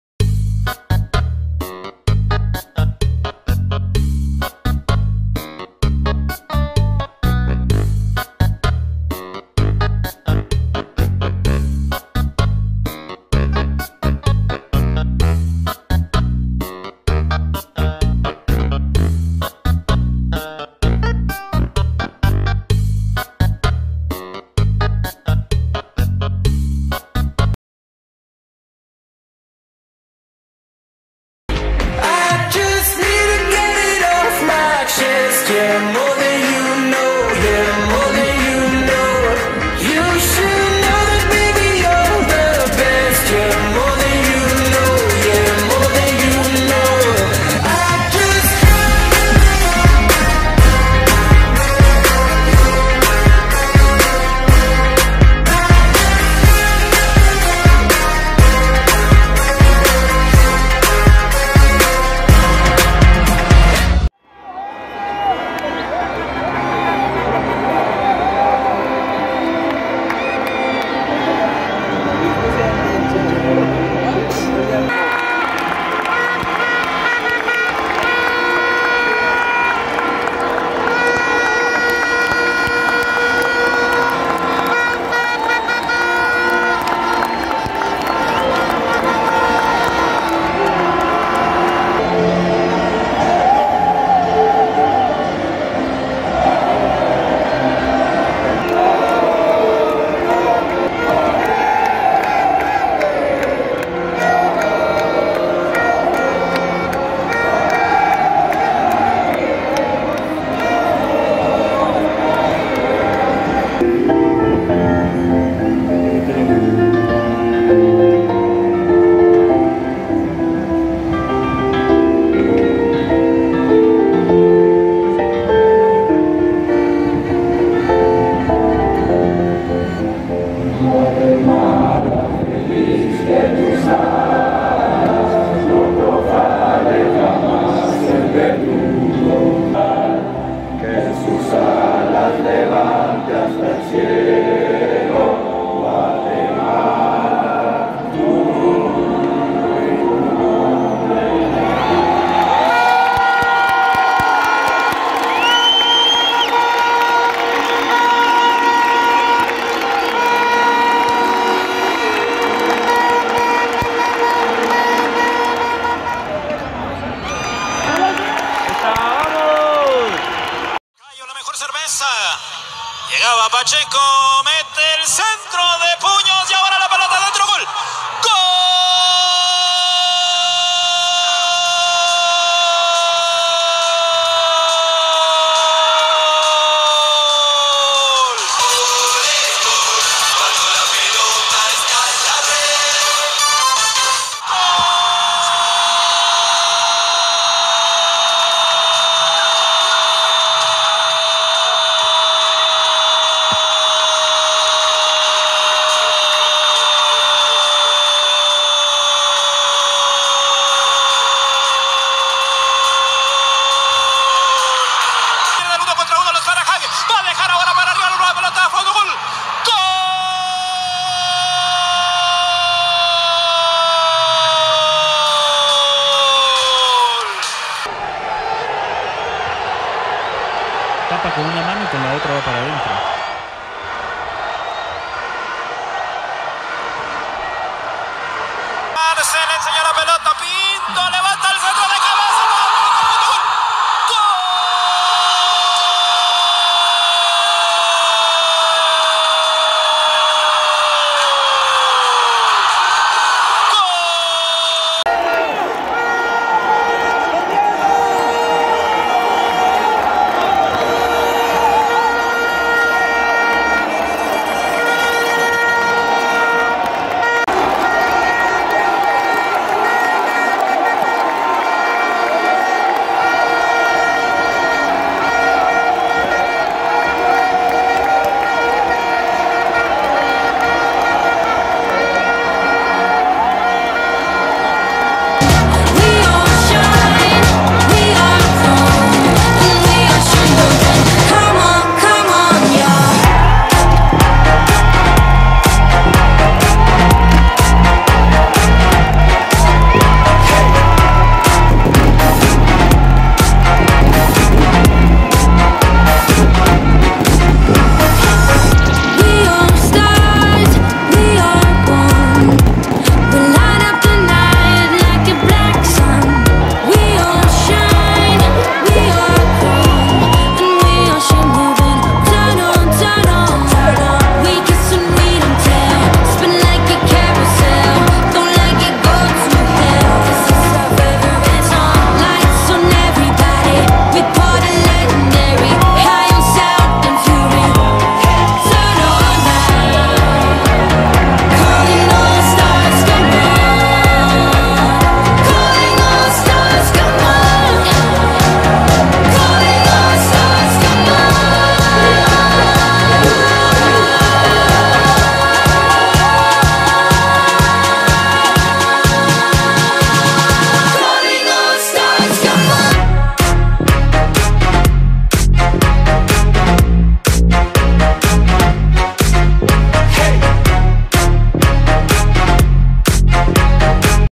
¡¡MUNICIPAL CAMPEON DE LA LIGA NACIONAL!!-vlog Final Torneo Apertura
Muy buenas chicos estamos aqui en nuevo video en el que hoy les traigo
un vlog de la final del torneo apertura 2019 espero que les guste y que todos